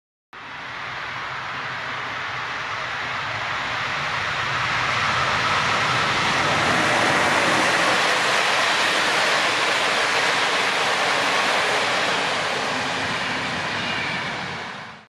Egy 7 kocsis Napfény InterCity halad Szeged felé
Mozdony pályaszáma: 431 129
Kocsik típusa: Bd, Bd, Bd, Bd, Bd, Bpee, ARmz,
A vonat Budapest-Nyugati pu. - Szeged viszonylatban közlekedett.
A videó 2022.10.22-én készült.